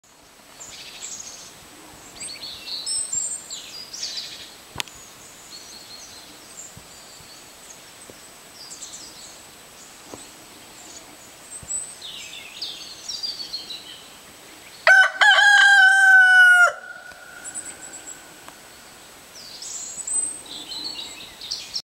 Rooster crowing from the Czech Republic - part V.
Nedělní ráno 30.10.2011. Sousedův kohout už je na našem území. Naše slepice jsou ještě zavřeny v kurníku.